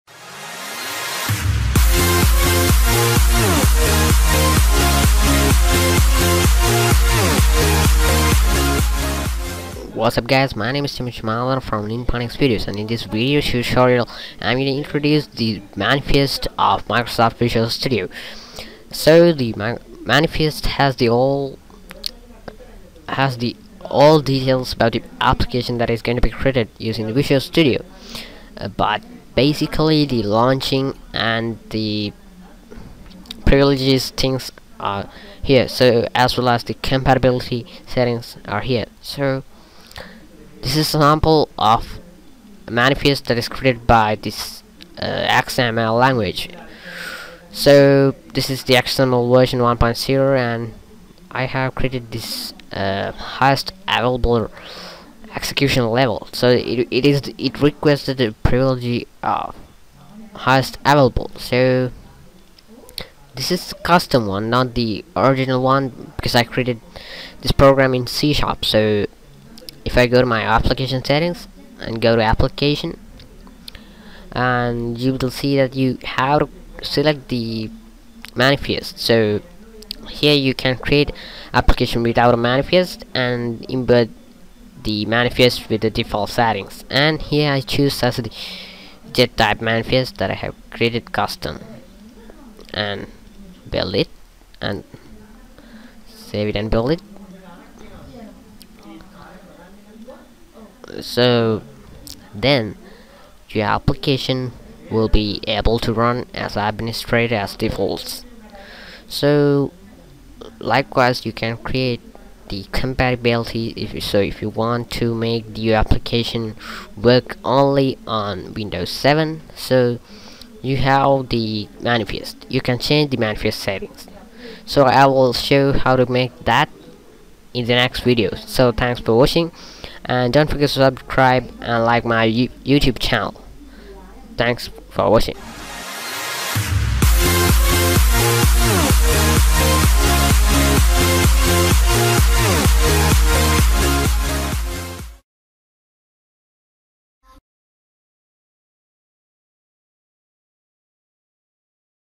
Create Custom Manifest in C# Visual Studio
Hello Friends, I'm Chamuth Chamandana from Ninponix (CEO of Ninponix). Today, in this video tutorial I'm gonna teach you how to create a custom manifest in C# and to add it to the current project.

INTRODUCTION
You may know that the Manifest (.manifest) file inside the .EXE file or inside the Path of the installation is the container of the metadata of the program that you have created with C#. You can do many Platform related and environment related conditional launches with manifest. ex:- Changing UAC Level.

Ninponix in Facebook, Google +
Chamandana in Twitter